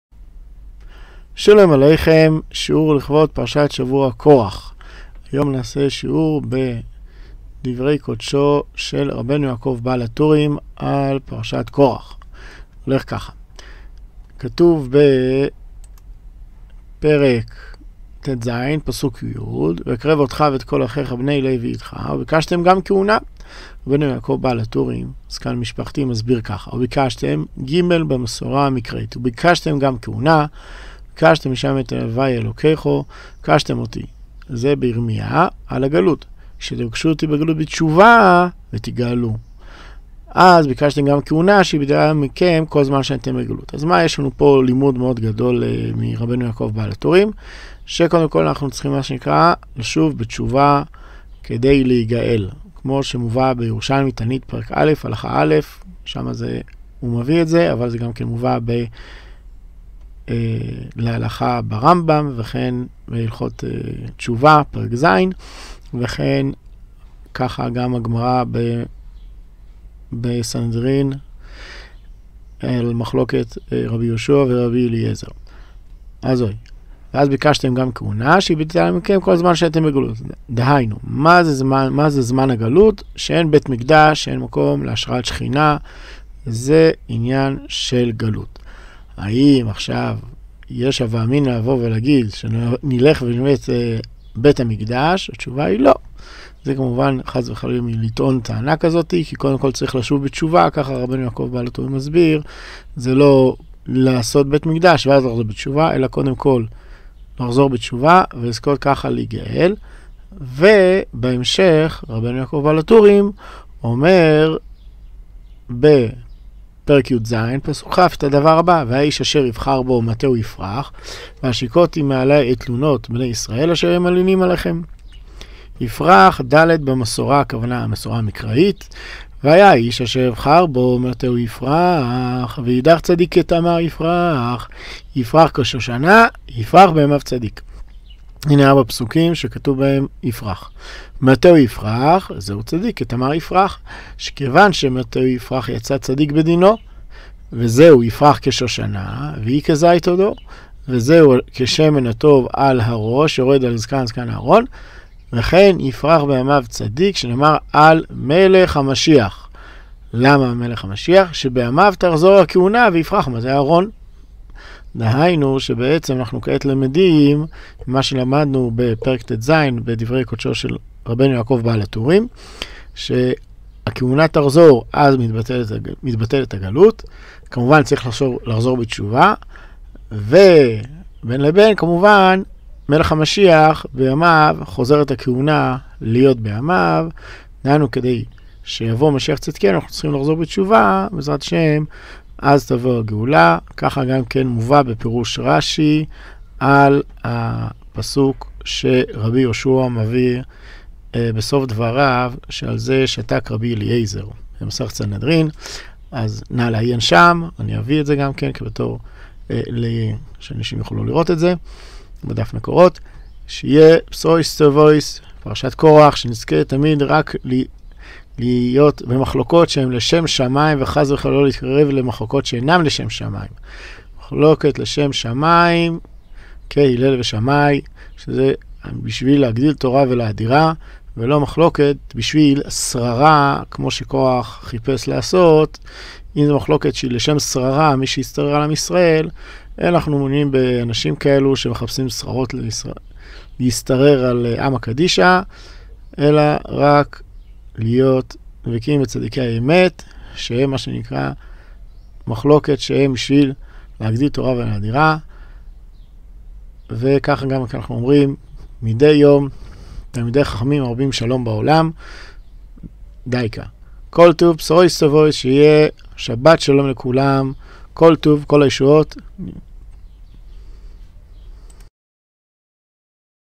שיעור מוקלט לכבוד פרשת קרח ה'תש״פ, ביאור בדב"ק של רבינו יעקב בעל הטורים זיע״א
לתרומה ותמיכה בפעילותינו
* דרך IBAN א - IL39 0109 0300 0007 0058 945